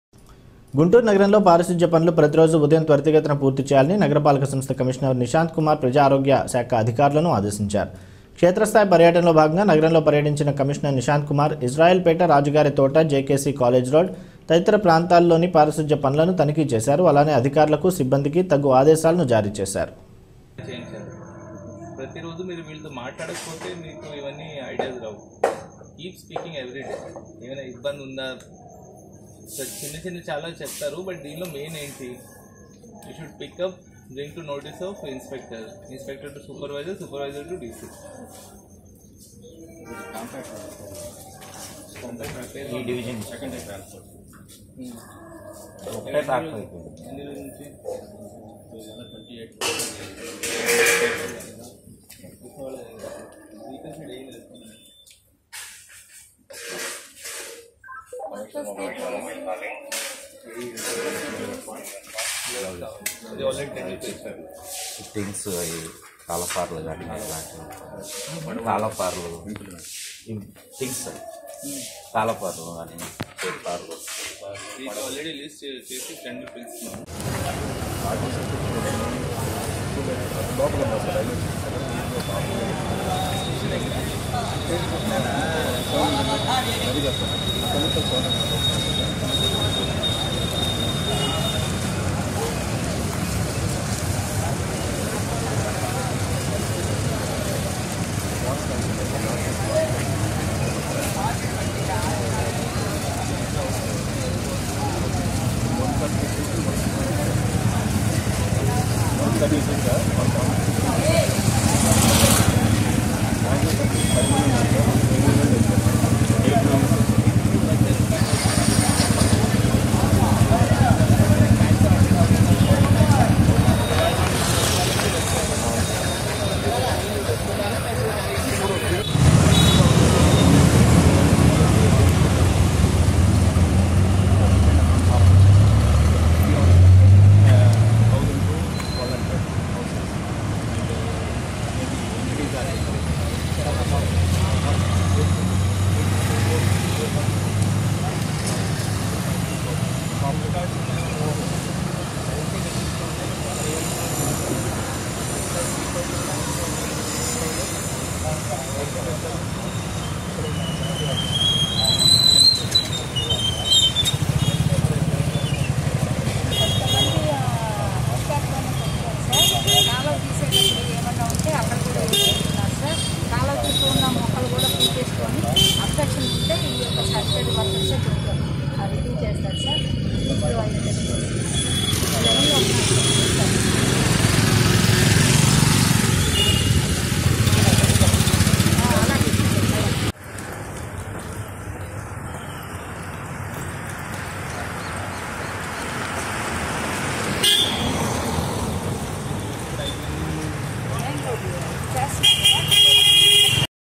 గుంటూరు నగరంలో పర్యటించి పారిశుద్ధ్య పనులను పరిశీలించిన కమిషనర్ నిశాంత్ కుమార్..
గుంటూరు నగరంలో పారిశుద్ధ్య పనులు ప్రతిరోజూ ఉదయం త్వరితగతిన పూర్తి చేయాలని నగరపాలక సంస్థ కమిషనర్ నిశాంత్ కుమార్ ప్రజారోగ్య శాఖ అధికారులను ఆదేశించారు. క్షేత్రస్థాయి పర్యటనలో భాగంగా నగరంలో పర్యటించిన కమిషనర్ నిశాంత్ కుమార్.. ఇజ్రాయెల్ పేట, రాజుగారి తోట, జె.కె.సి కాలేజి రోడ్ తదితర ప్రాంతాల్లోని పారిశుద్ధ్య పనులను తనిఖీ చేశారు. అలానే  అధికారులకు, సిబ్బందికి తగు ఆదేశాలను జారీ చేశారు.